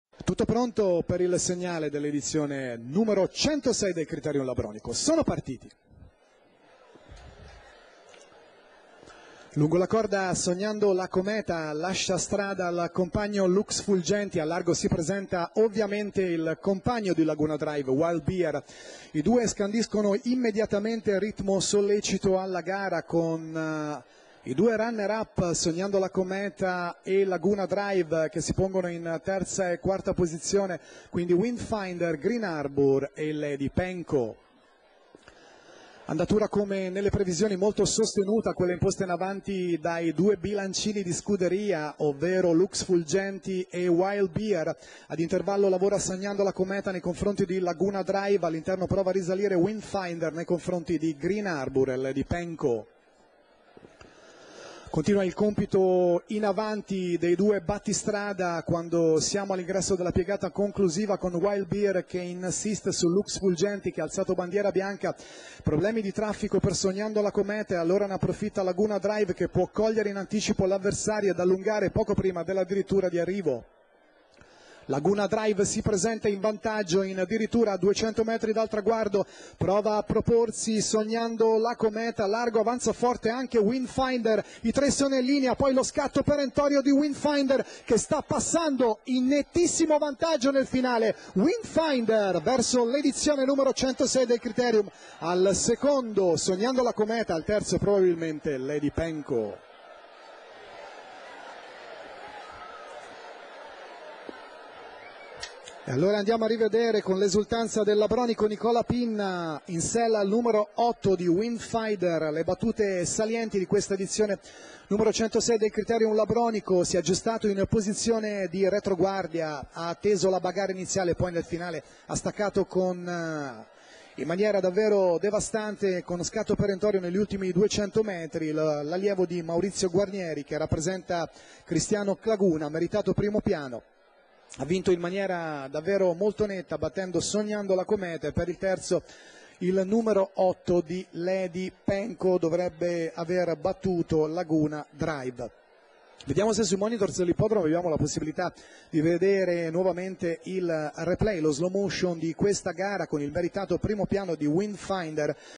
11/08/2013, Livorno , Premio 106� Crit.labronico
1. WINDFINDER (PINNA NICOLA), 2. SOGNANDO LA COMETA (LANDI STEFANO), 3. LADY PENKO (SULAS S.), 4. LAGUNA DRIVE (FIOCCHI CARLO), 5. GREEN HARBOUR (LAMBERTI C.), 6. WILD BEAR (KOLMARKAJ M.), 7. LUX FULGENTI (SANNA MARIO)